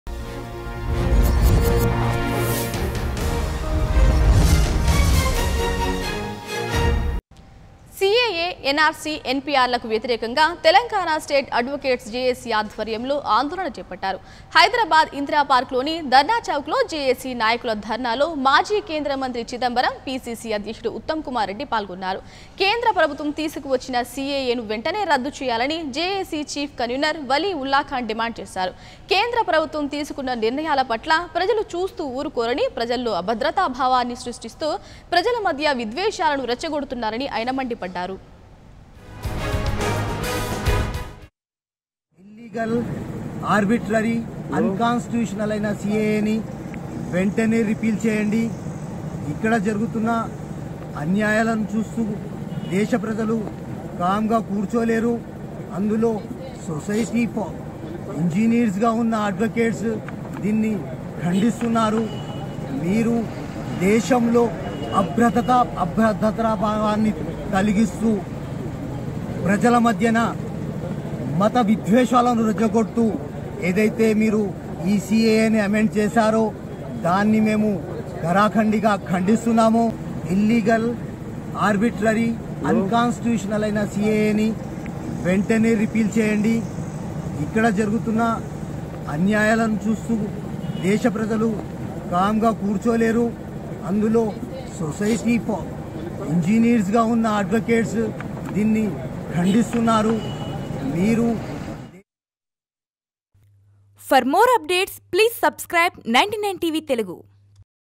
Chidambaram And Uttam Kumar Reddy Joins With Telangana Advocates For CAA, NRC Protest | 99 TV Telugu
Chidambaram And Uttam Kumar Reddy Joins With Telangana Advocates For CAA, NRC Protest.